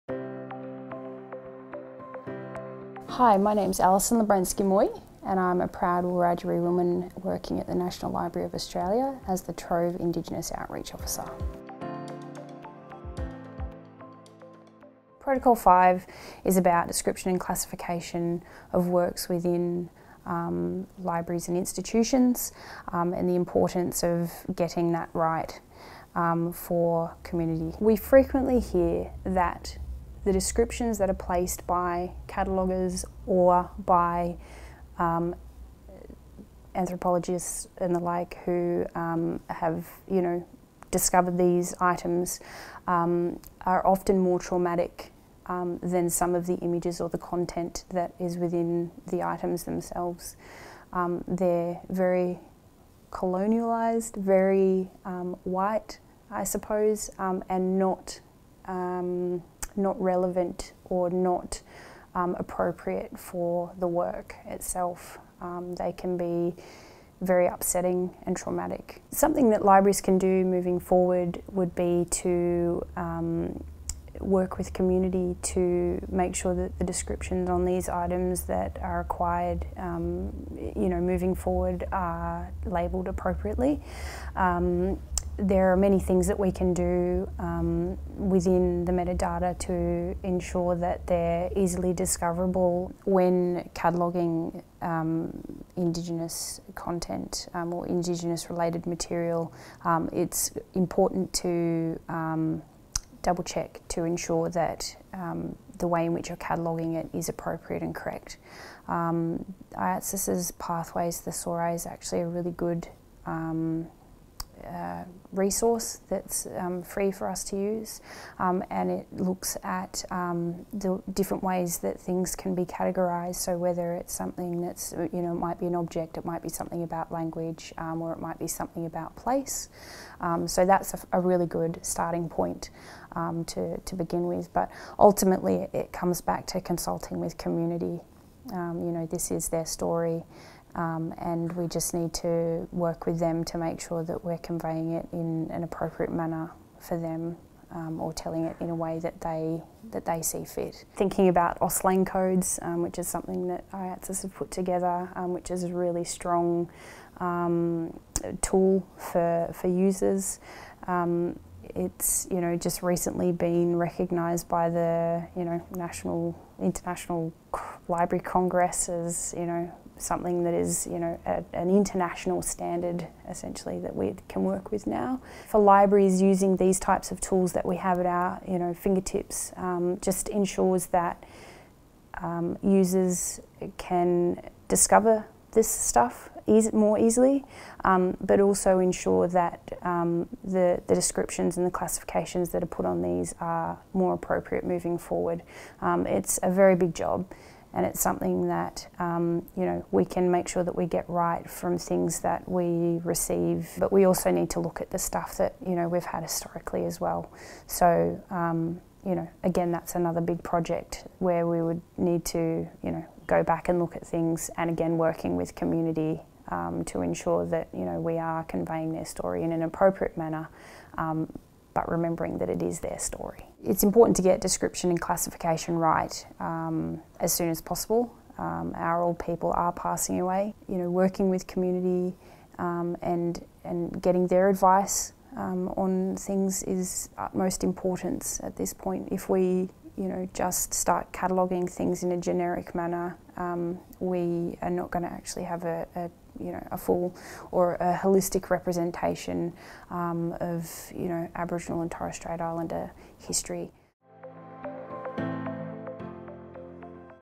ATSILIRN Protocol 5: Description and Classification
Allison Lubransky-Moy, Trove Indigenous Outreach Officer at the National Library of Australia, talks about the importance of applying appropriate metadata to Aboriginal and Torres Strait Islander collection items.

Learn more about the ATSILIRN Protocols and how they can be applied when working with Indigenous collections